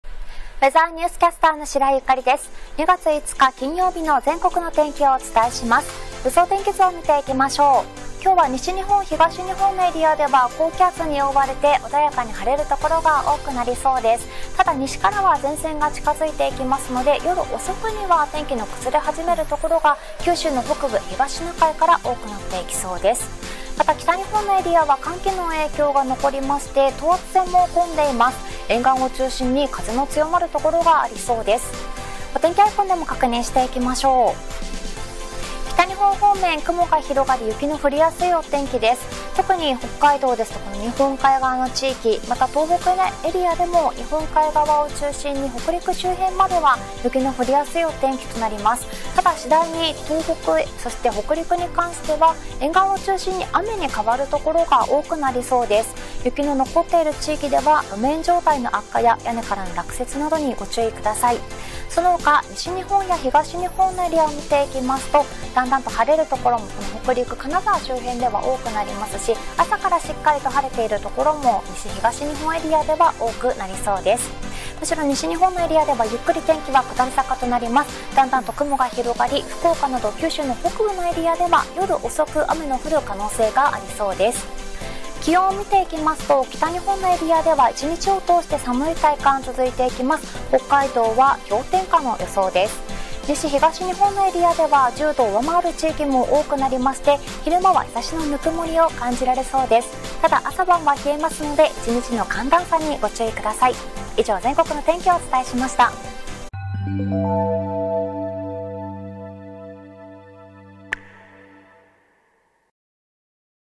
お天気キャスター解説 2月5日(金)の天気
おはようございます。今日は高気圧に覆われて、西・東日本の太平洋側では晴れる所が多くなります。
ただ、西から天気は下り坂で、夜遅くは九州北部で雨が降り出す予想です。
北陸は午前ほど雪の降る可能性がありますが、段々と天気は回復。
北日本は日本海側で、雪が降りやすく、東北では次第に雨寄りに変化していきます。
西・東日本では、朝晩は寒いですが、昼間日差しの温もりを感じることができそうです。
北日本は寒さが続きますので、暖かくしてお過ごしください。

今日は笑顔の日＾＾
マスクの下でもニコッと笑って、お顔の筋肉も動かしてあげましょう！
それでは、今日も元気にいってらっしゃい♪

解説：ウェザーニュースキャスター 白井ゆかり